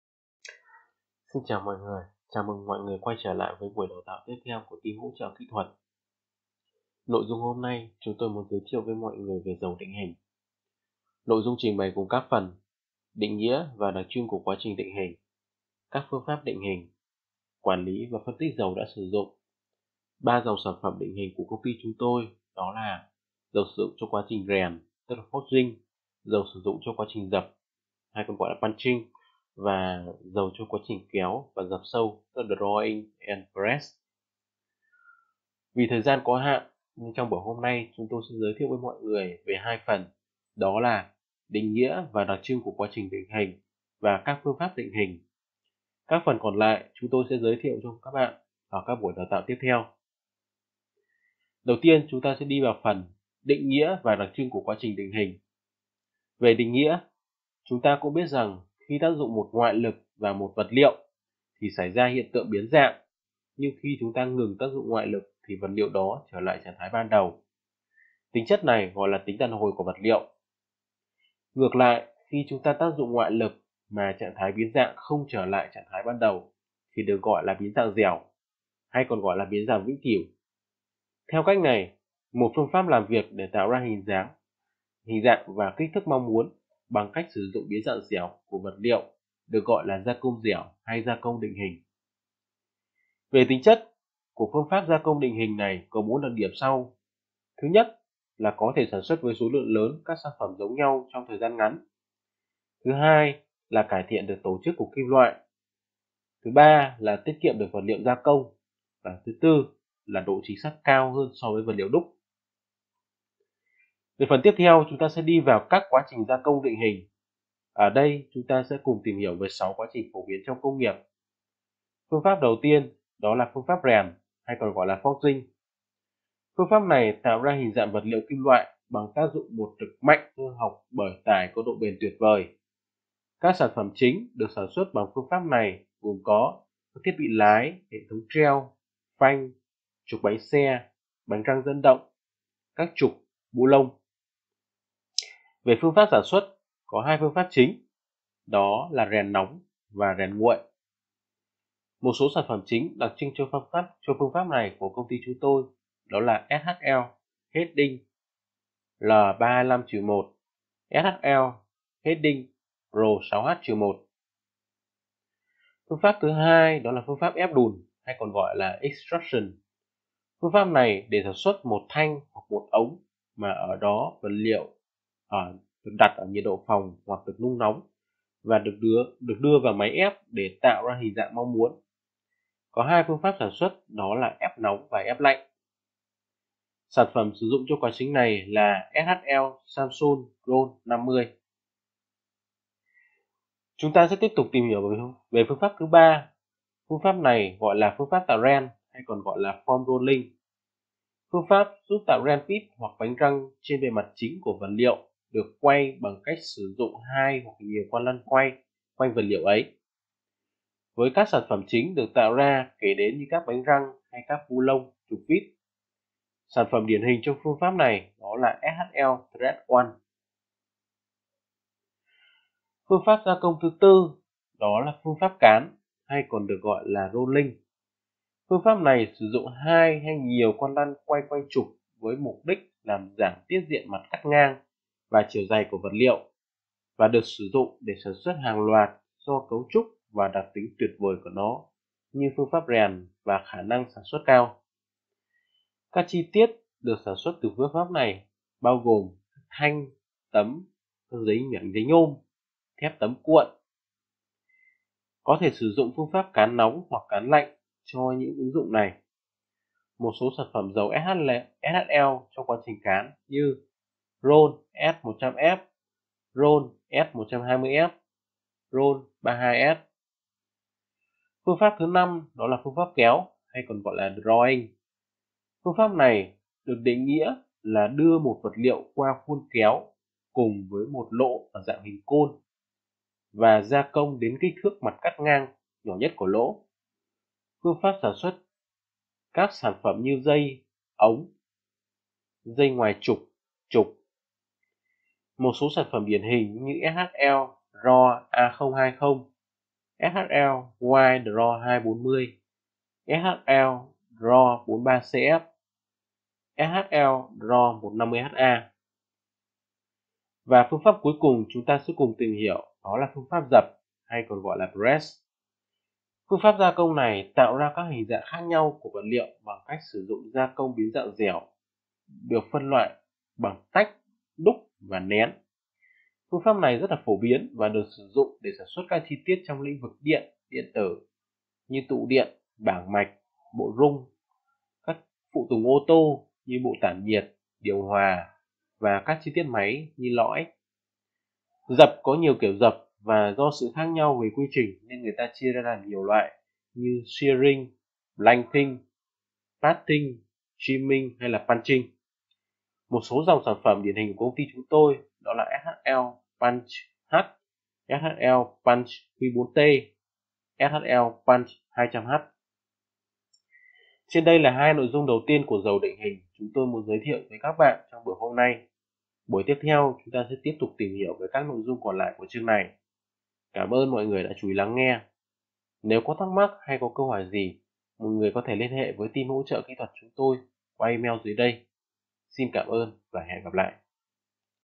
[SHL ASEAN] Đào tạo kỹ thuật Chương 4-1: Giới thiệu về Dầu định hình [Forming oil]
Definition and characteristics of plastic working and types of plastic working.
Định nghĩa và đặc trưng của quá trình định hình và các phương pháp định hình.